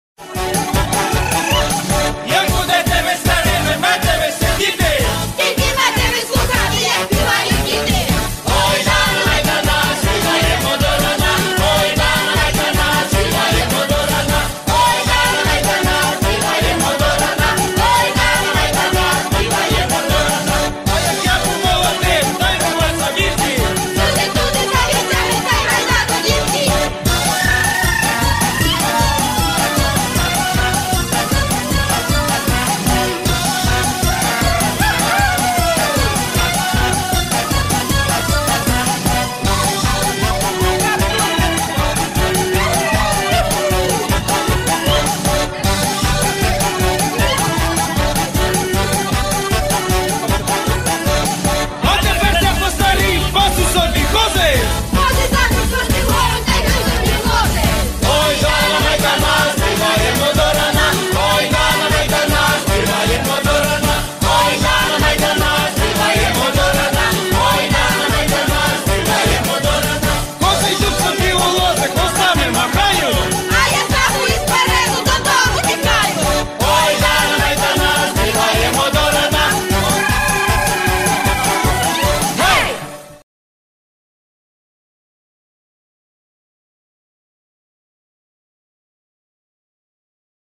Веселий Вулик 2007 рік.
Народний аматорський фольклорний колектив "Веселий Вулик"
Керівник Світлана Рибальська 2007 рік.Збірка фото.